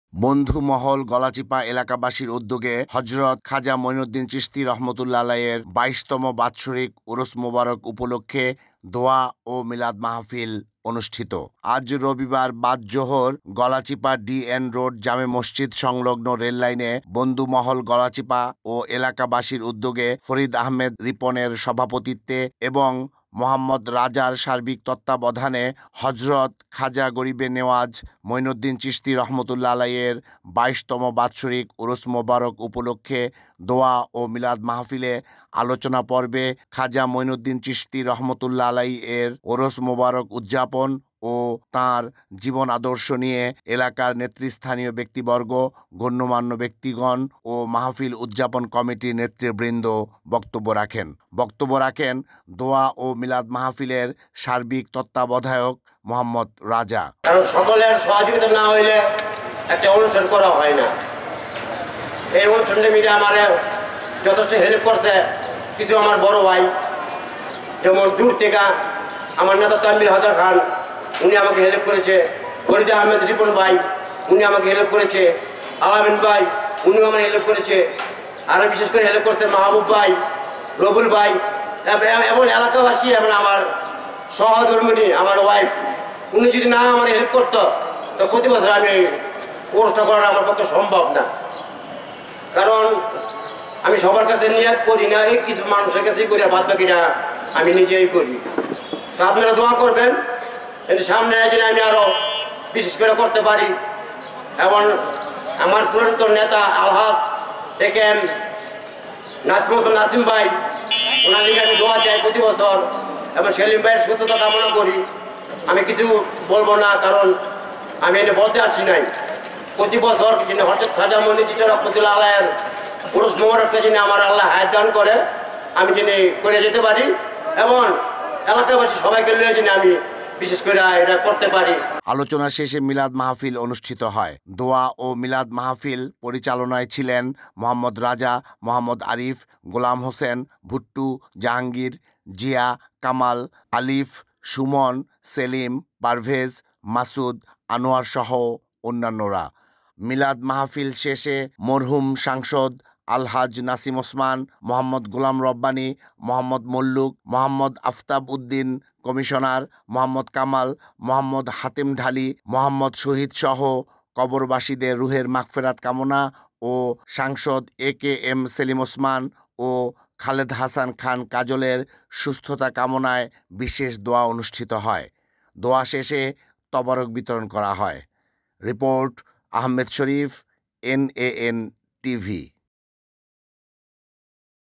গলাচিপায় বন্ধুমহলের উদ্যেগে খাজা মঈনউদ্দিন চিশতী (রঃ) এর ওরশ উপলক্ষে দোয়া ও মিলাদ মাহফিল || NAN TV
গলাচিপায় বন্ধুমহলের  উদ্যেগে খাজা মঈনউদ্দিন চিশতী (রঃ) এর ওরশ উপলক্ষে দোয়া ও মিলাদ মাহফিল

Official Address:
76/2 Uttor Chashara, Rajdhani Apurbo Tower,